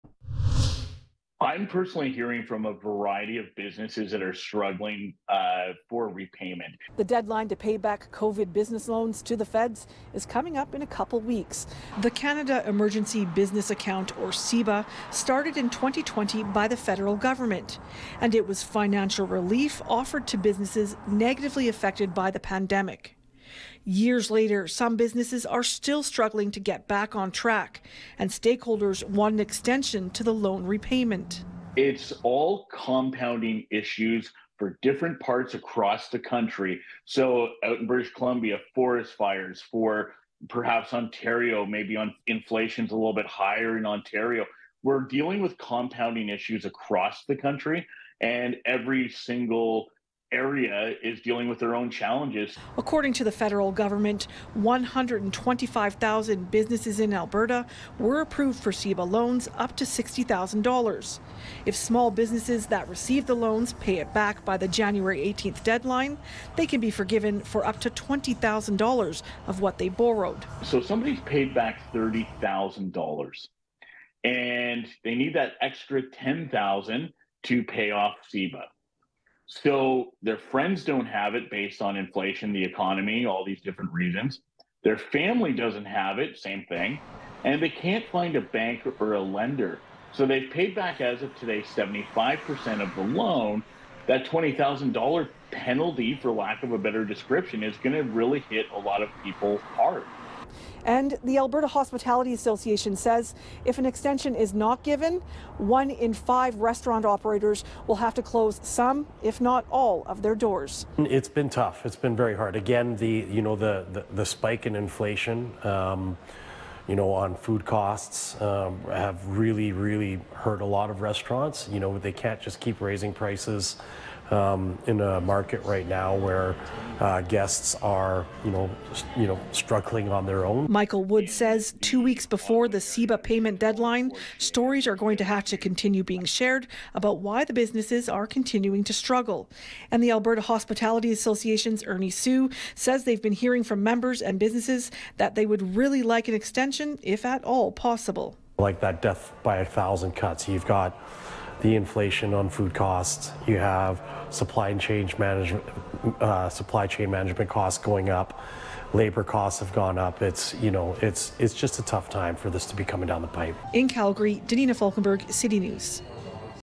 Stakeholders want CEBA loan repayment deadline extended
It’s almost time for small businesses that received relief loans during the pandemic to pay back the Feds. Danina Falkenberg learns this has small business advocates fighting for an extension to the payment deadline.